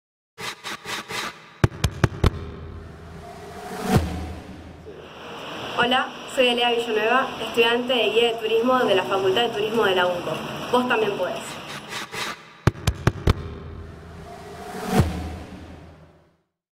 Ingreso 2020 en la Universidad Nacional del Comahue
UNCo Iingreso 2020 - A partir del 1 de noviembre de 2019 estará habilitada la "Preinscripción Online" para el Ingreso 2020 en la UNComa